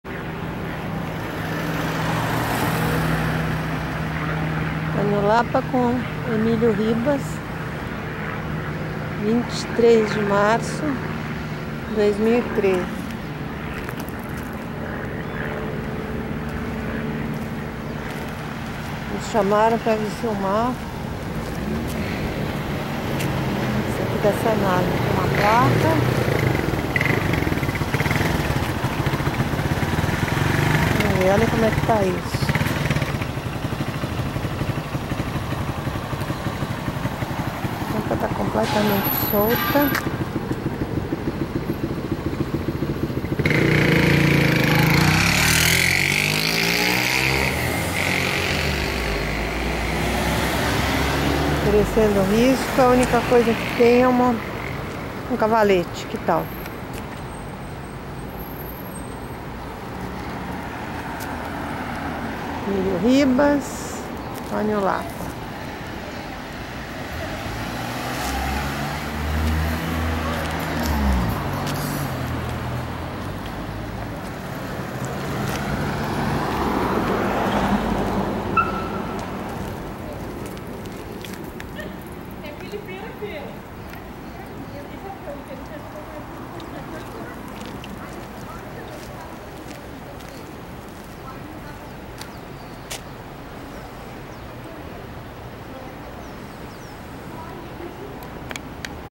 Cratera Sanasa no Cambui
Cratera no cruzamento das ruas Emilio Ribas e Antonio Lapa, em Campinas
Data 23/3/15

Moradores pedem para ser documentada a cratera formada em volta de uma tampa completamente solta da Sanasa, podendo causar grave acidente.Confira no video.